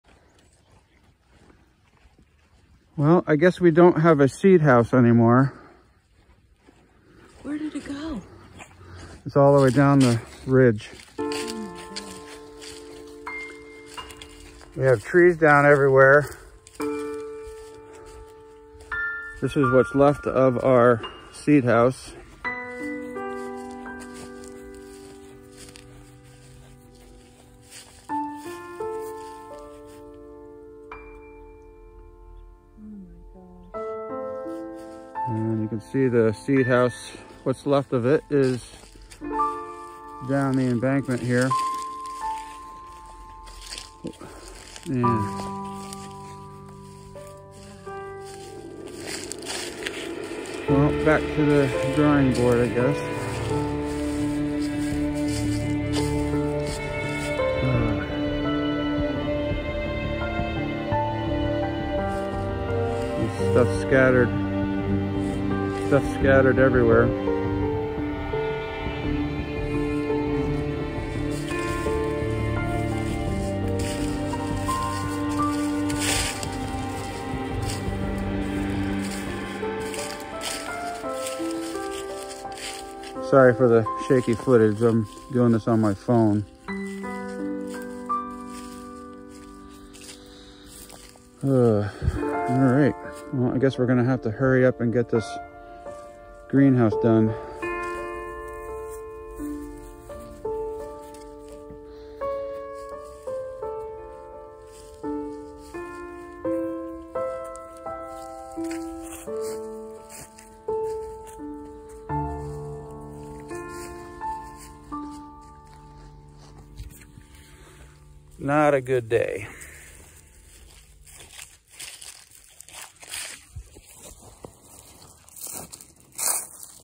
Our Seed House Is Gone!
We had extremely strong winds in our area today. The power was out and knocked down trees... and now we just discovered that our seed house is gone. Not a good day on the homestead.

Thanks again!
                                Jay & Cheryl